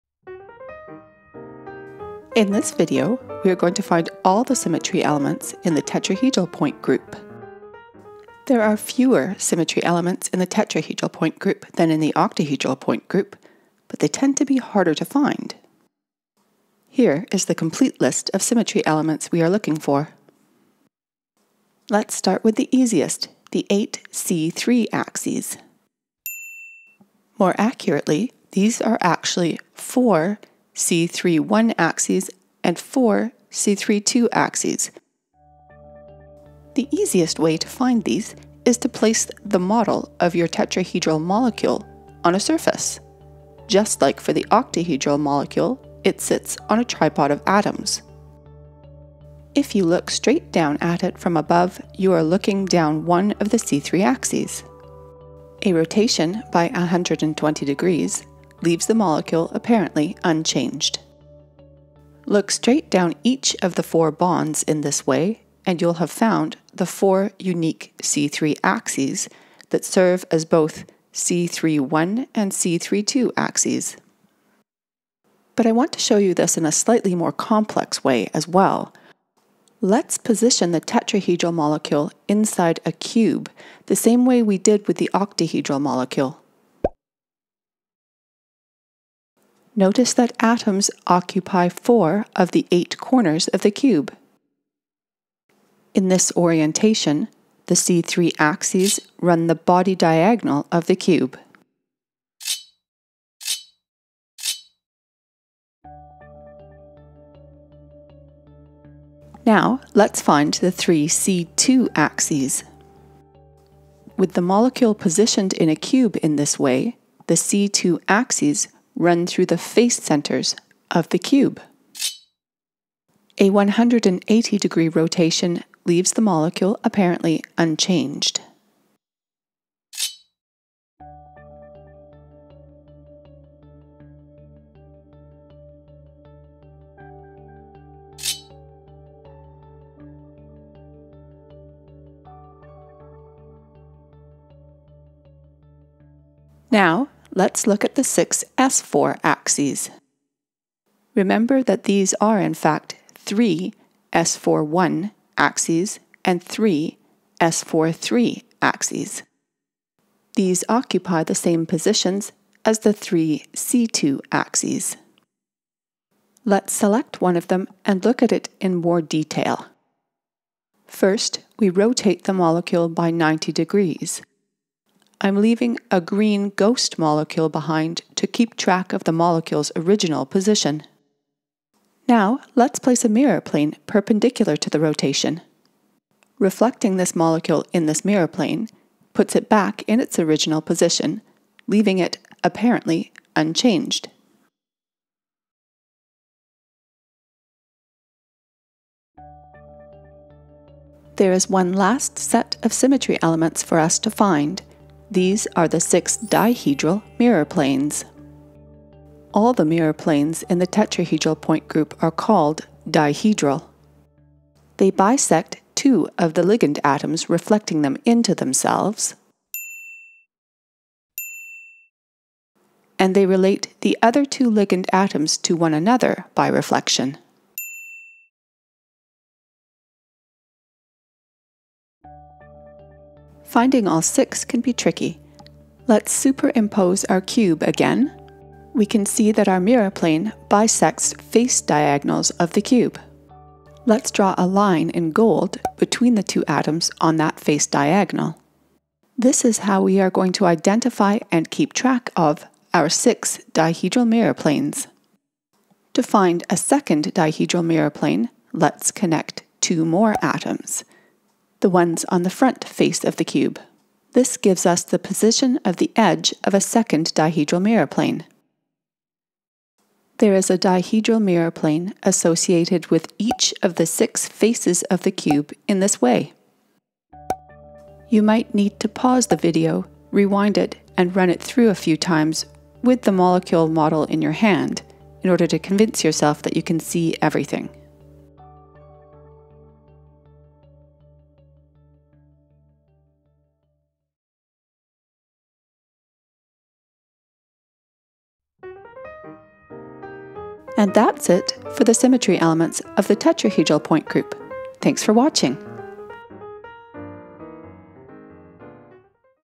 Td symmetry elements
Here we look at each of the symmetry elements in the Td point group. Grab your molecular model kit and follow along!